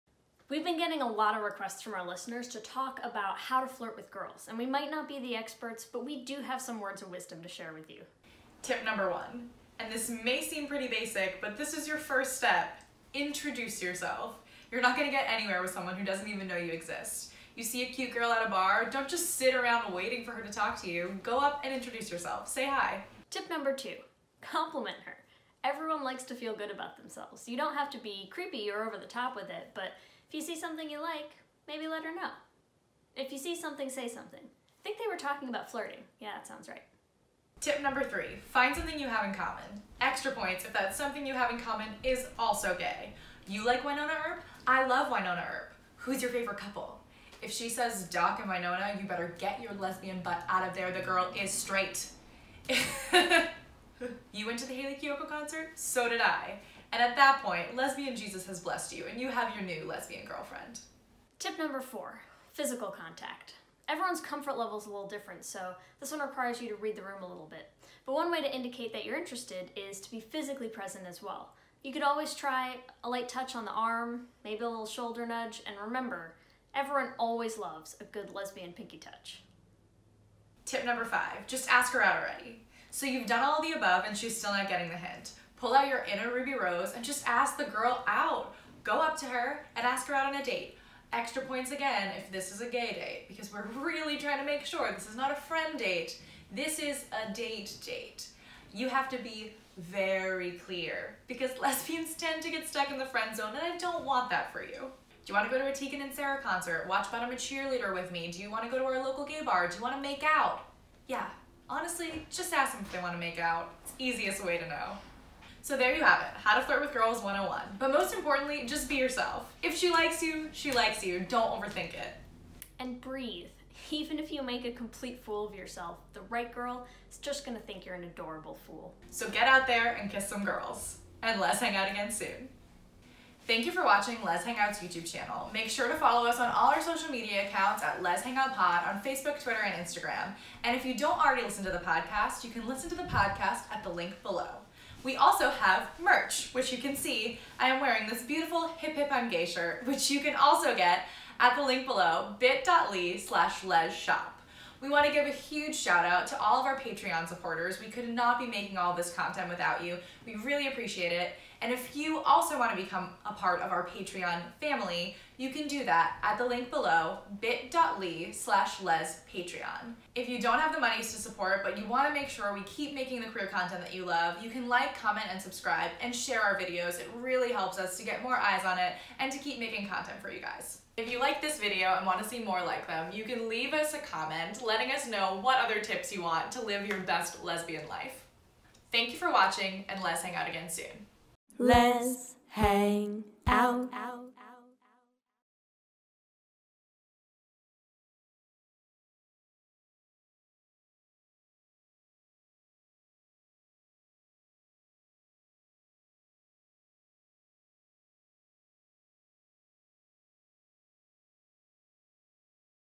How To Flirt With Girls: Tips from Lez Hang Out
You've been asking us about how to flirt with girls, so here are our top 5 tips!

COMMENT with other tips you want us to give so you can live your best lesbian life